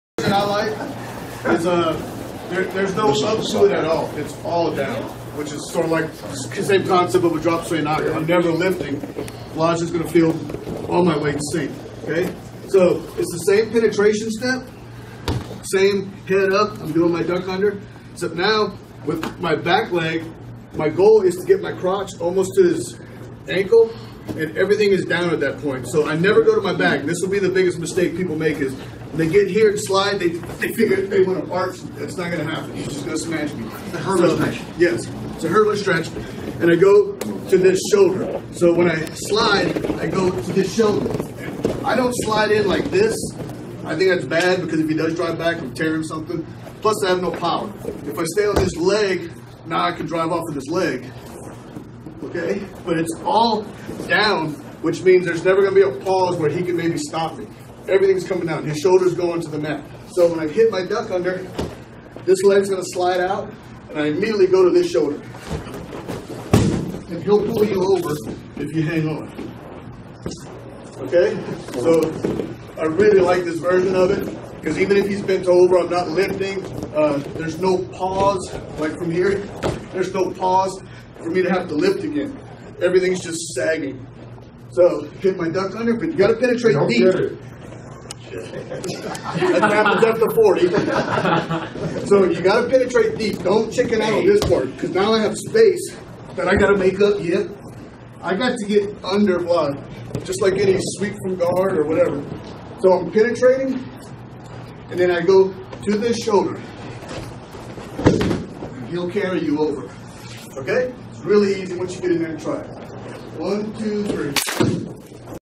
Coach Doug Fournet Demonstrating Fireman's Carry Variant
борьба САМБО

Chief Instructor: Patrick Kennedy
School Name: Kennedy's Martial Arts Academy
School Location: Elk Grove Village, IL